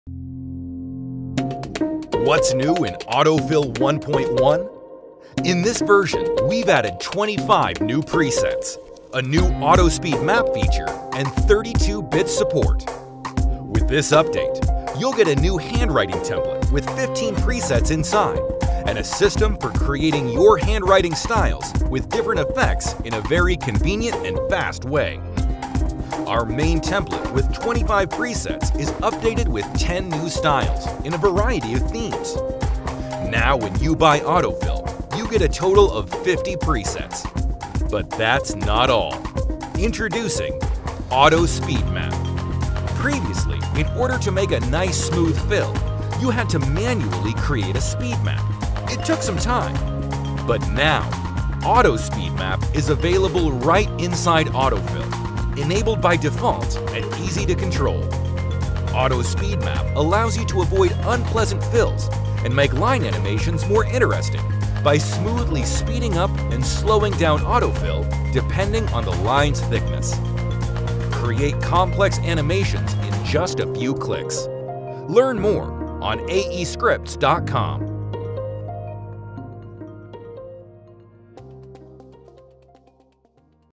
Autofill - More Speed, More Power, More Features - Version 1.1 Update - After Effects Plugin
We are here today to announce an update to the amazing AutoFill! Version 1.1 comes with More Speed, More Power, and More features!

AutoFill is an intelligent write-on and reveal plugin that follows your lines and layers perfectly.

No more Keyframes, and thousands of masks, it is time for one simple drag-and-drop tool with 25 built-in presets! What are you waiting for, get your fill of AutoFill!

SOCIAL